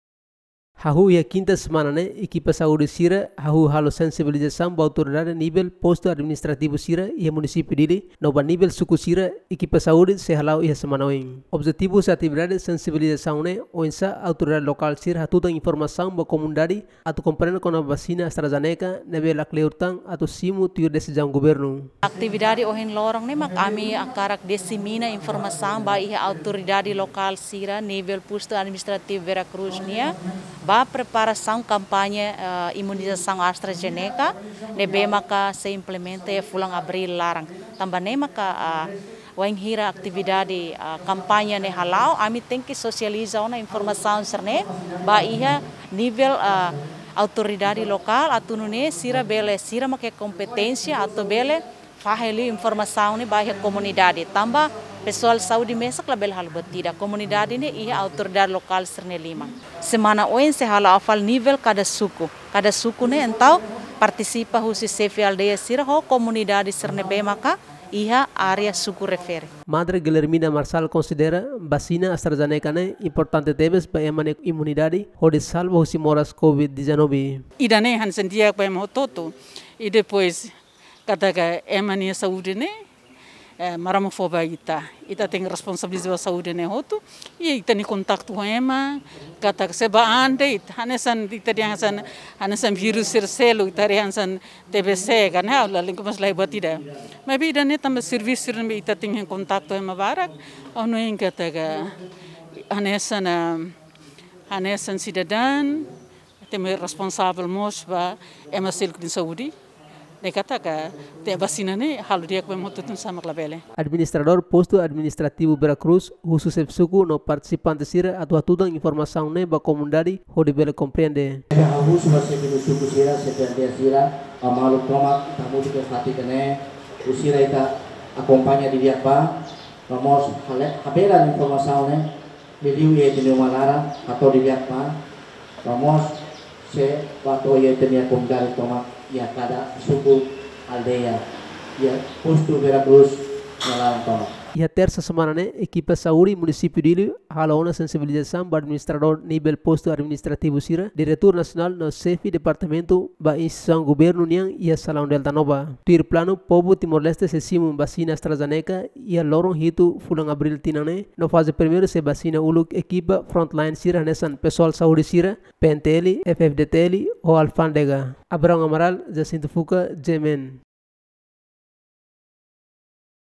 Ekipa Saúde hala’o sensibilizasaun vasina AstraZeneka ba Autoridade Lokal Vera Cruz
Ekipa Saúde hala’o sensibilizasaun ba Autoridade Lokal sira iha Vera Cruz kona ba vasina AstraZeneka